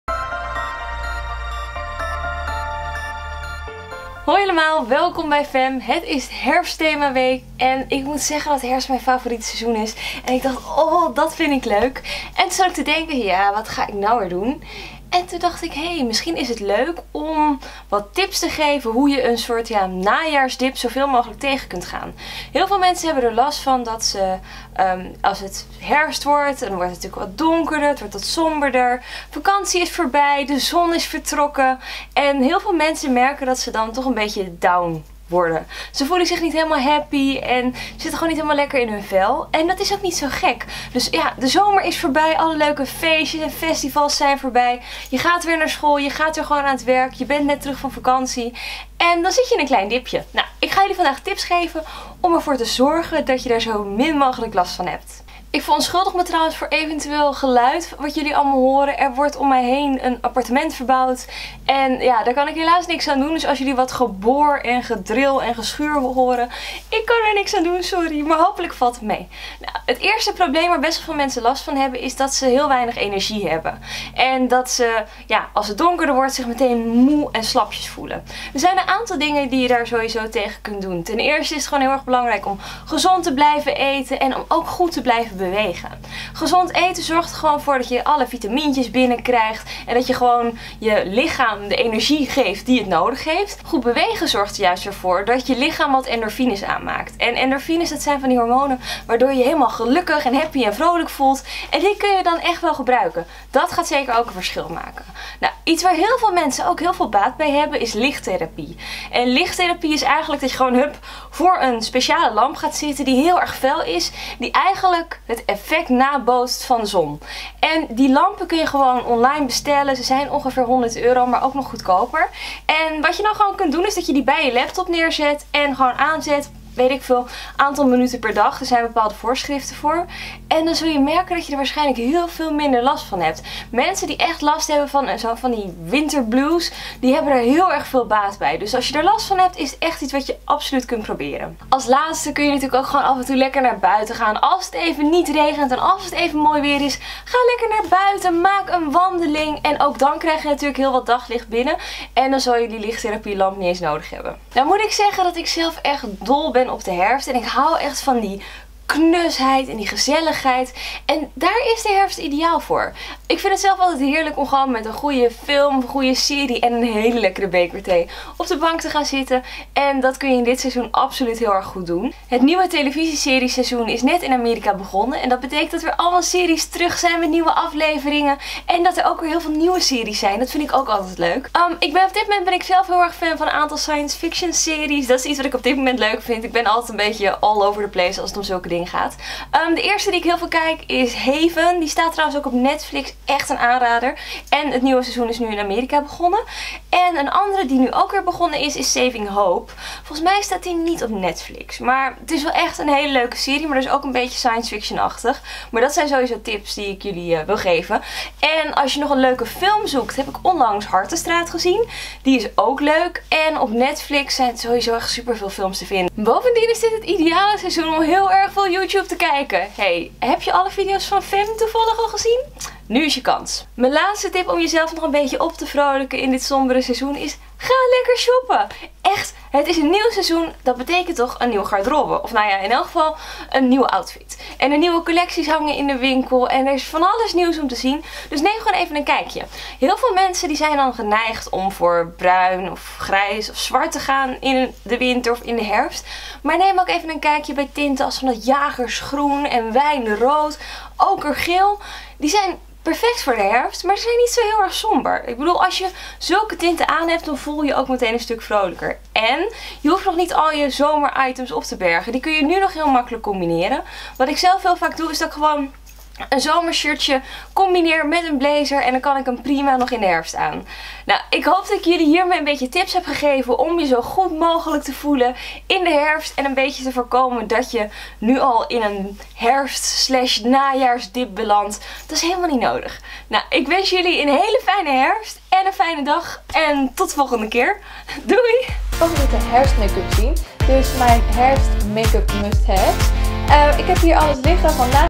Zo voorkom je een najaarsdip! - FEMME
Word jij in het najaar altijd wat somber en heb je last van het donkere weer? Ik geef in deze video tips hoe je zo'n najaarsdip na de zomer kun voorkomen. Enjoy! - Charlotte

VERWACHT OP DE VOLGENDE DAGEN EEN VIDEO:
MAANDAG: LOOKSBYSHARON
DINSDAG: LIFESPLASH
WOENSDAG: TODAYSBEAUTYNL
DONDERDAG: PINKYPOLISHNL
VRIJDAG: BEAUTYGLOSS